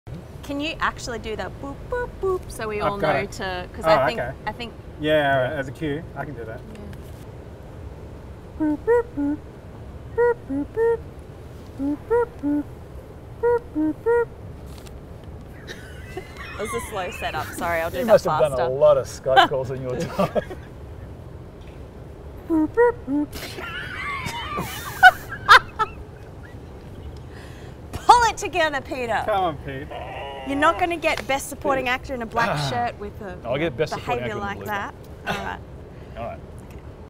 Making Critical Thinking Cafe: simulating a Skype call
Making a critical thinking video requires sophisticated movie magic. We may not have tennis balls on sticks but our state-of-the-art audio technology is second to none...